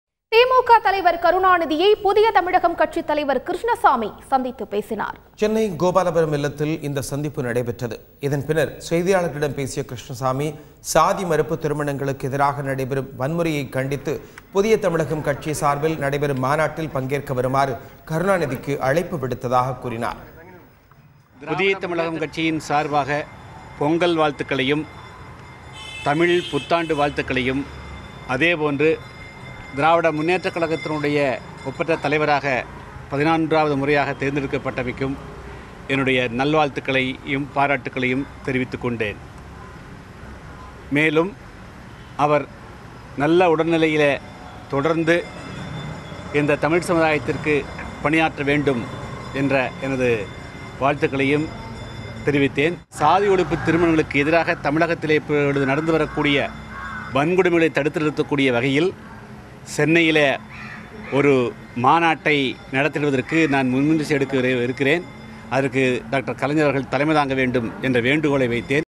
Puthiya Tamilagam Party Leader K.Krishnasamy meets DMK Chief Karunanidhi - Thanthi TV
Puthiya Tamilagam Party Leader K.Krishnasamy meets DMK Chief Karunanidhi to wish him well - Thanthi TV